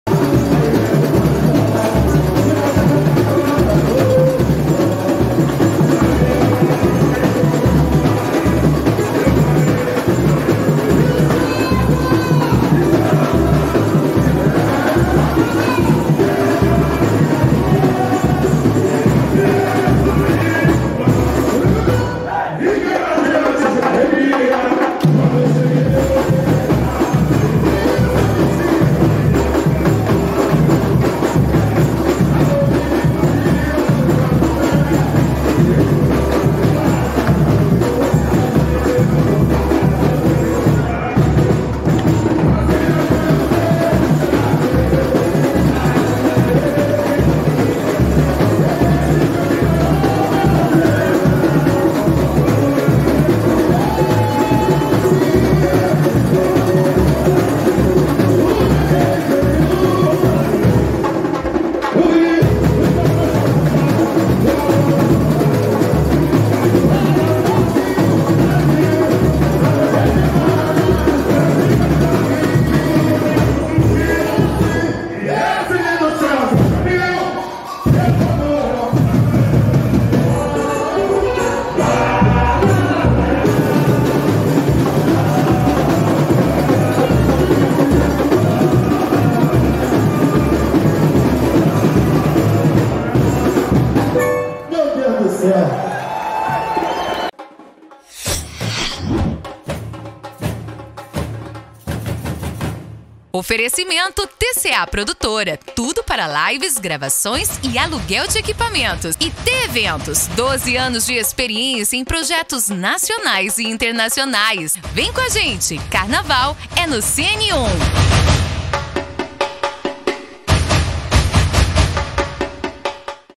Monique Rizzeto retorna ao Império Serrano para o Carnaval 2024
A influencer e empresária Monique Rizzeto está de volta ao Império Serrano. Ex-passista e rainha da escola da Serrinha, ela regressa à agremiação como princesa para brilhar no Carnaval 2024.

Monique chegou ao Império Serrano como passista. Depois, passou a reinar à frente da ala como madrinha, foi musa e rainha da escola, no Carnaval 2019.